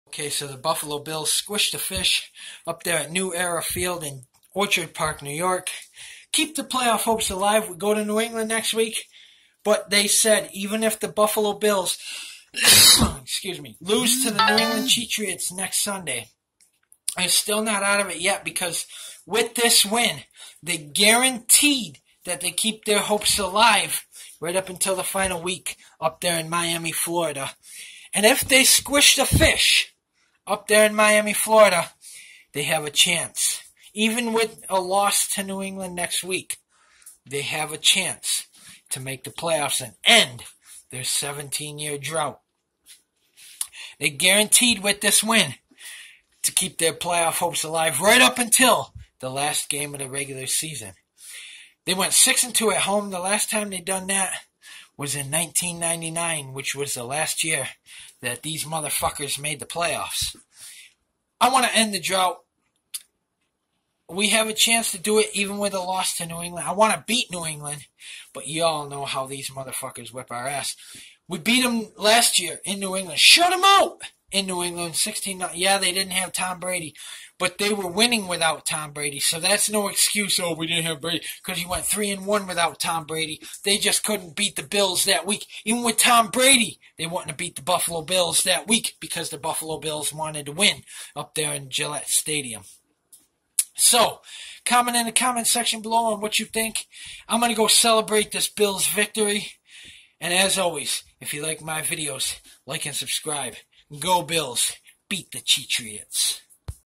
Bills squish the Fish 24-16. Bills stay alive in the playoff race
Bills win 24-16